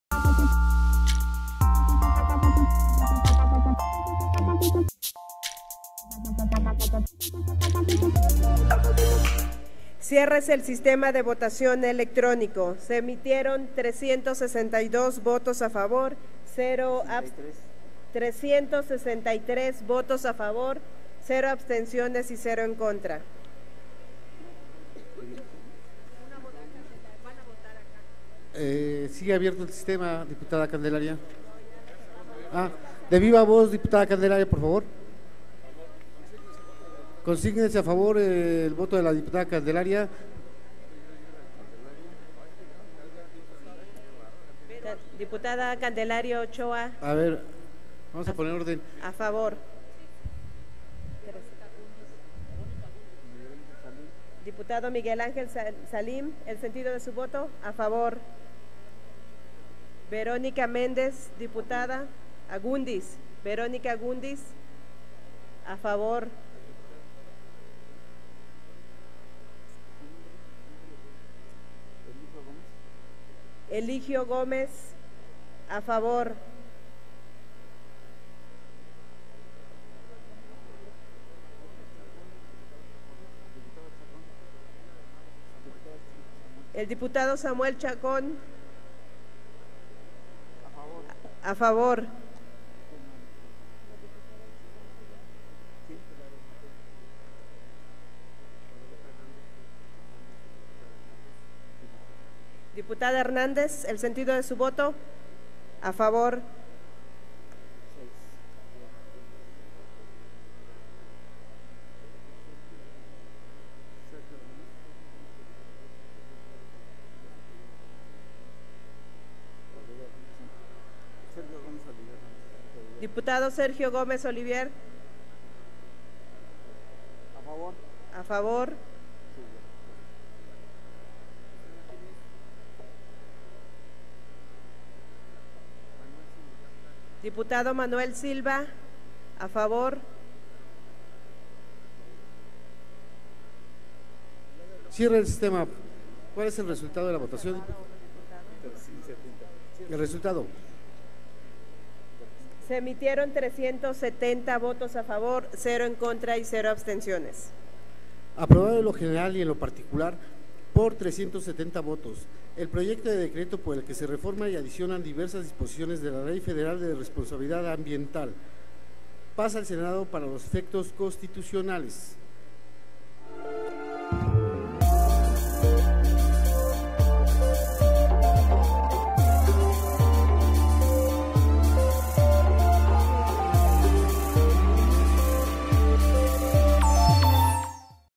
Aprobación Reformar la Ley Federal Responsabilidad Ambiental
En votación nominal se aprueba con 370 votos a favor, 0 en contra y 0 abstenciones, el dictamen por el que se reforma y adiciona diversas disposiciones de la Ley Federal de Responsabilidad Ambiental.
Aprobado en lo general y en lo particular el dictamen que reforma y adiciona diversas disposiciones de la Ley Federal de Responsabilidad Ambiental.